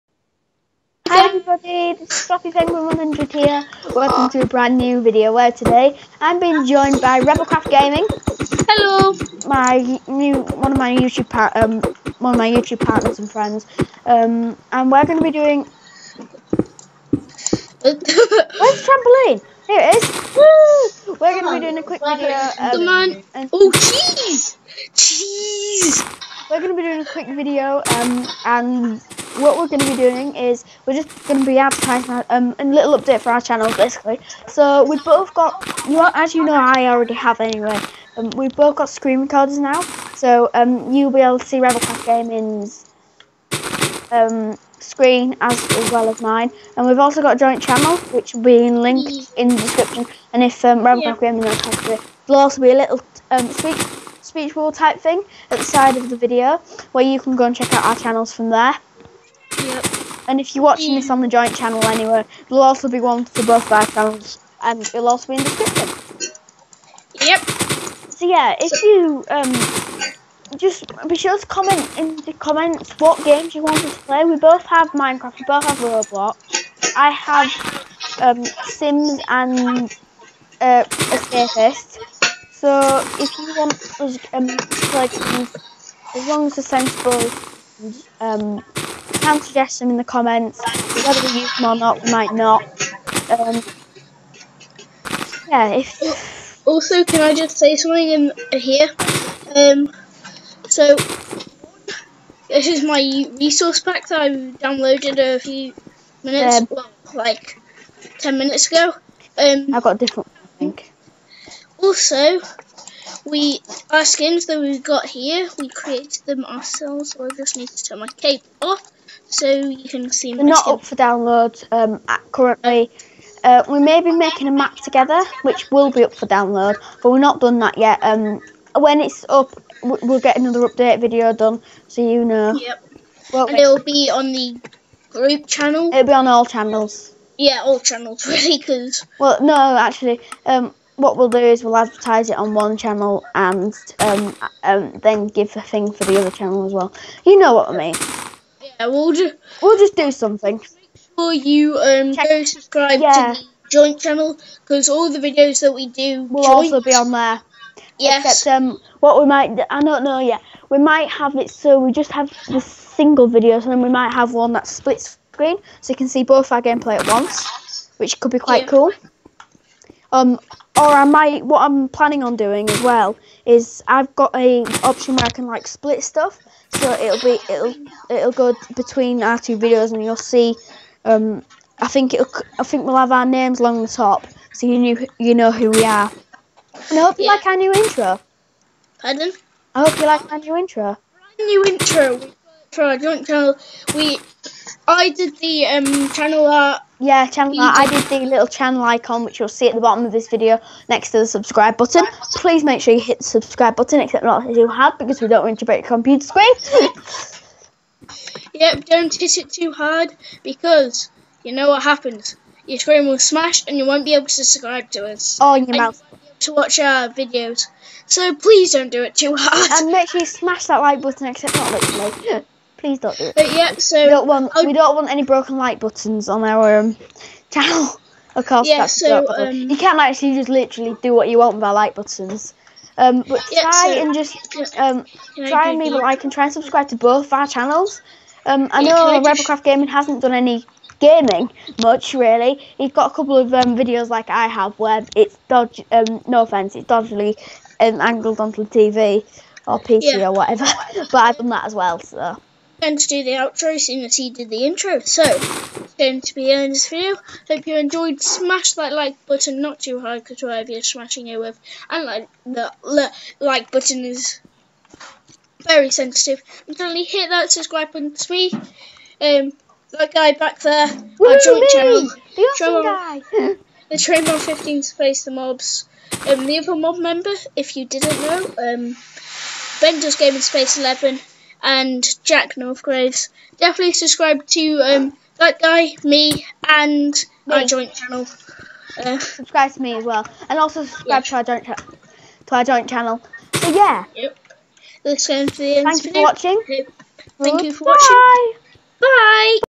Little update w/FlappyPenguin100
I am going to start making videos on the computer.

go check out: FP//RCG, FLappyPenguin100, TheTrainMan15 THEMOBS, Bendoesgaming 11 and Jack Northgraves